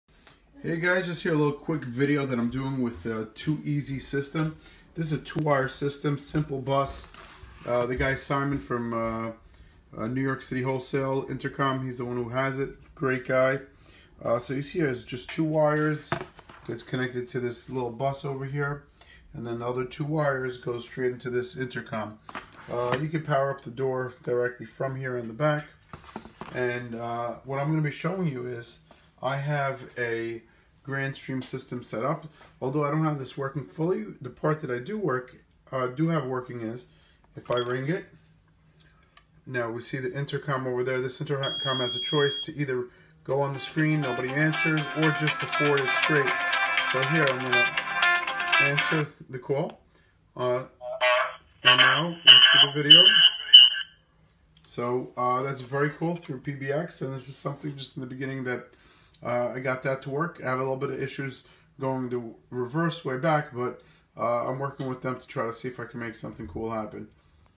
2-easy 2wire intercom registered on pbx
2 wire system connected to grandstream pbx
If you need purchasing info reach out